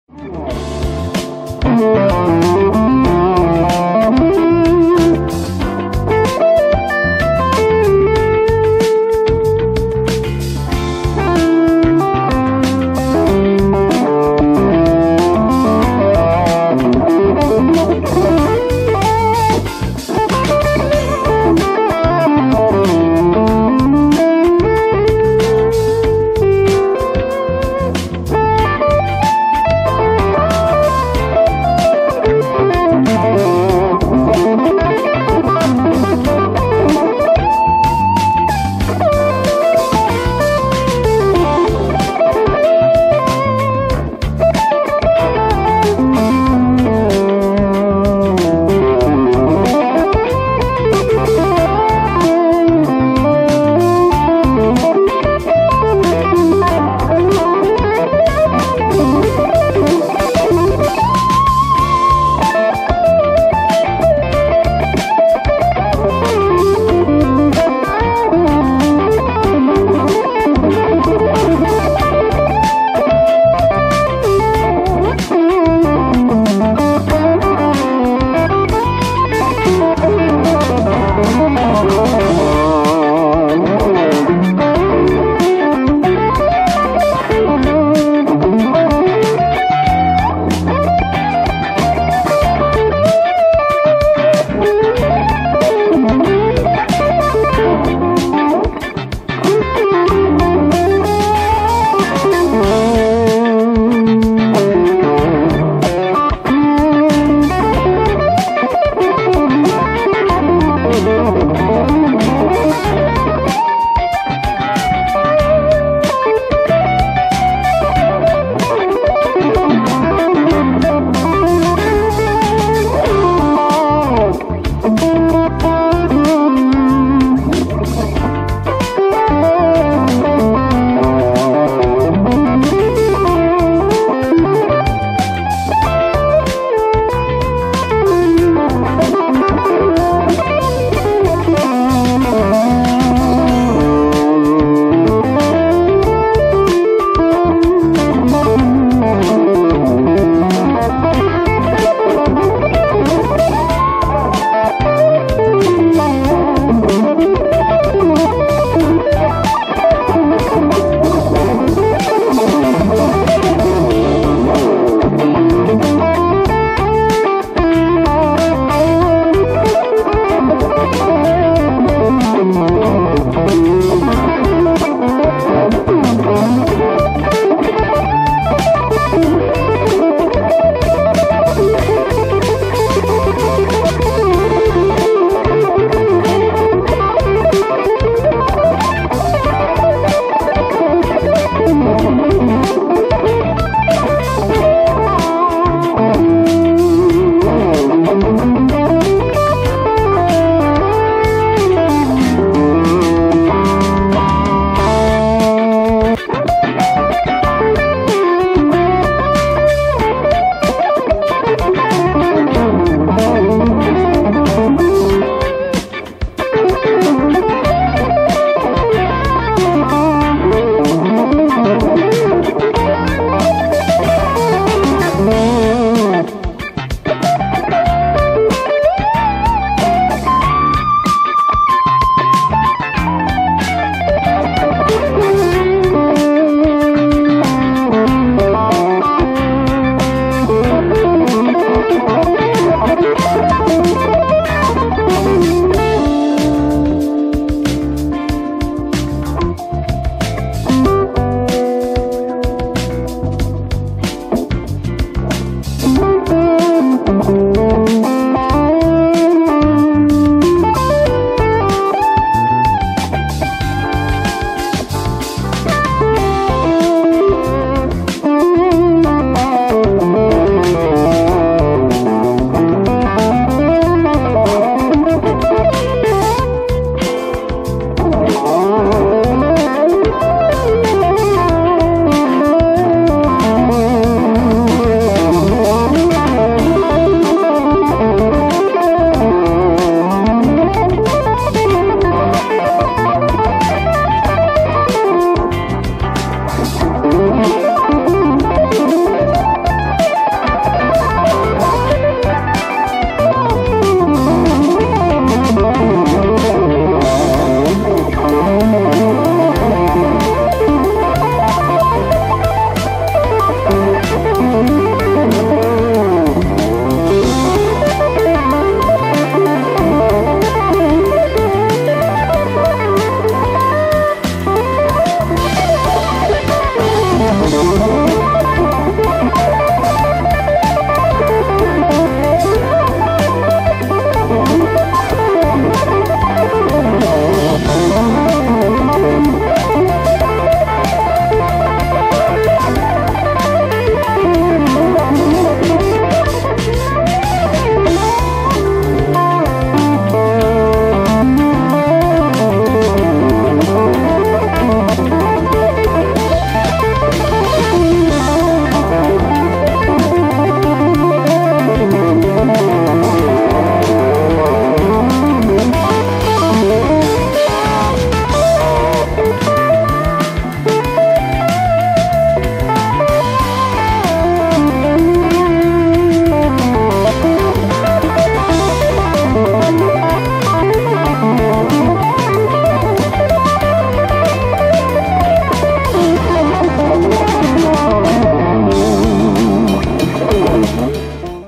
Improvisation for rehabilitation 2022.8.1.
術後のリハビリの為のピロピロ
Gear : Carvin HH2,  Cube Baby, Mooer Radar